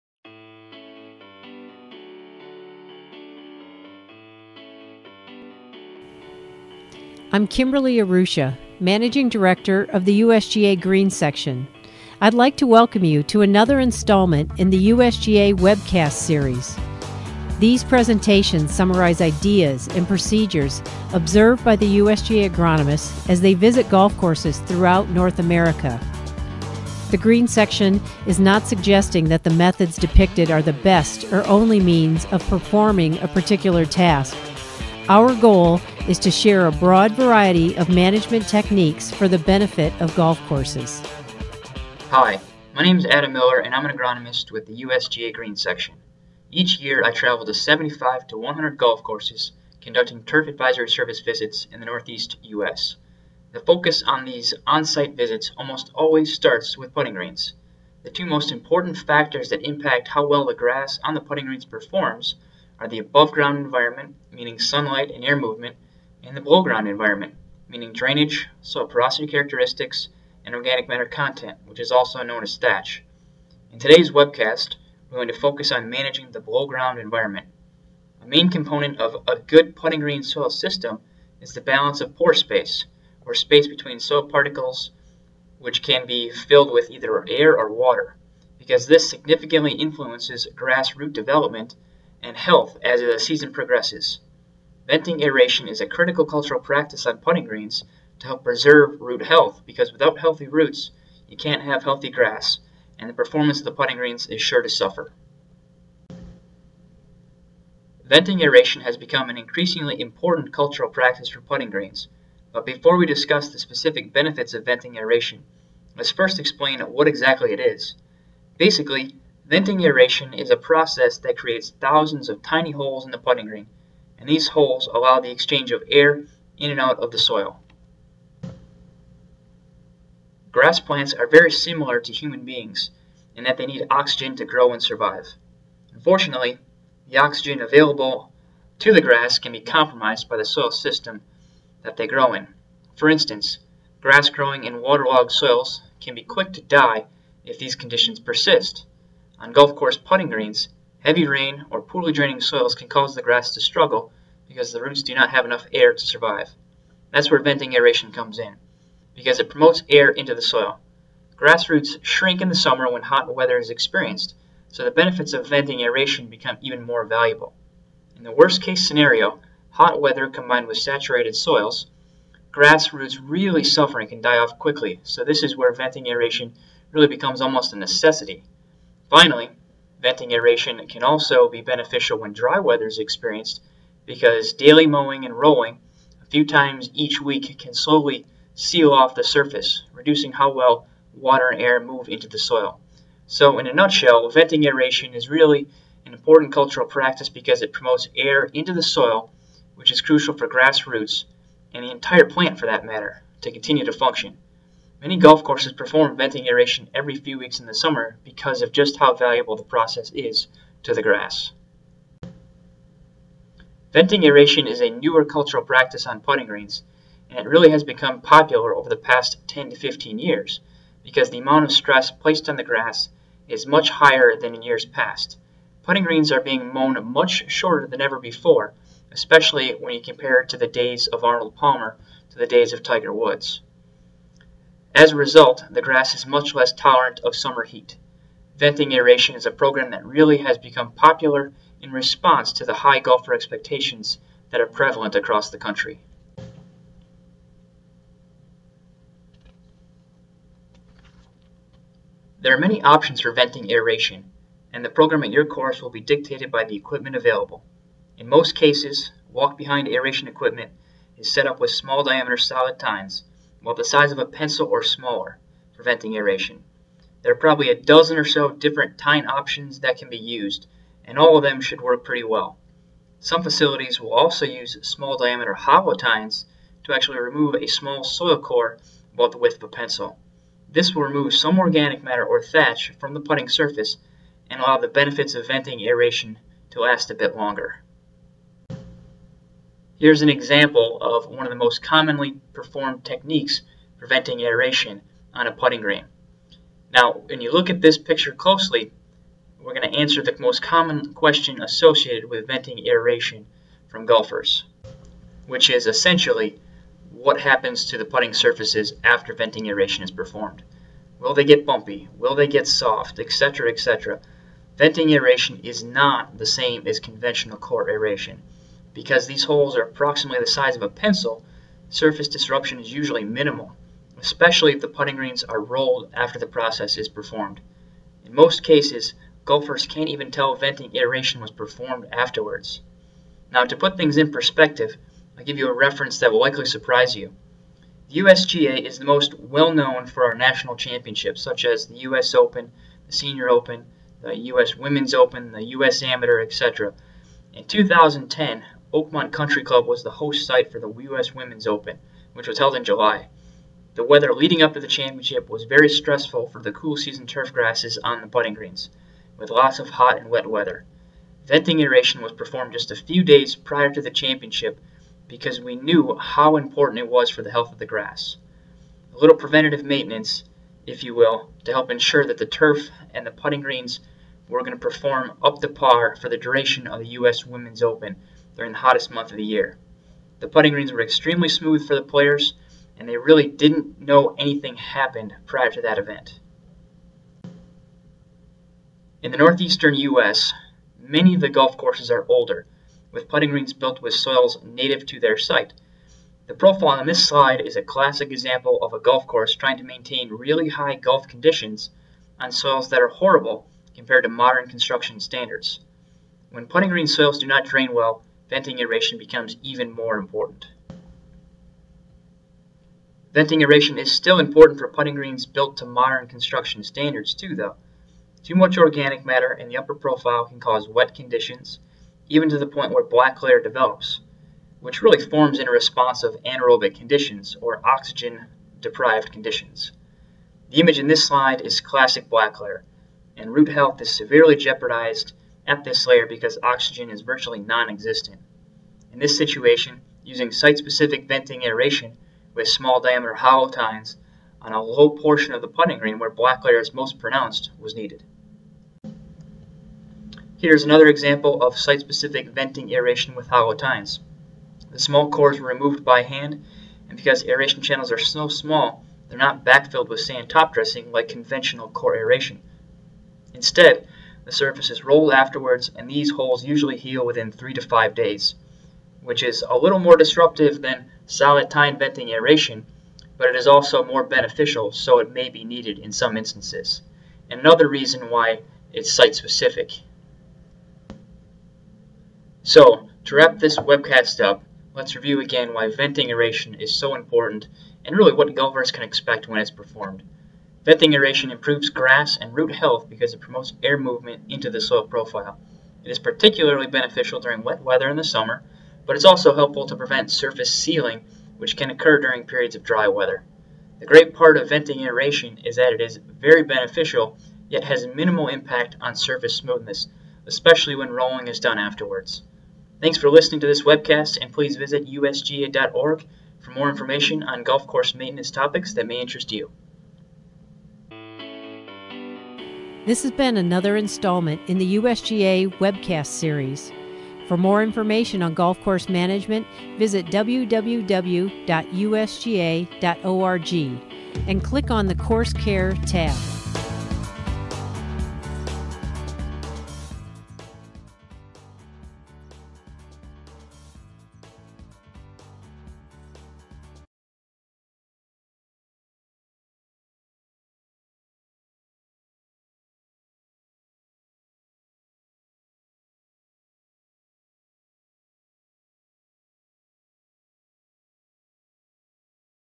Webcast: Venting Aeration Is A Benefit To Putting Greens
By Adam Moeller, agronomist, USGA Green Section Northeast Region.

Originally published in the 9/6/13 edition of the Green Section Record.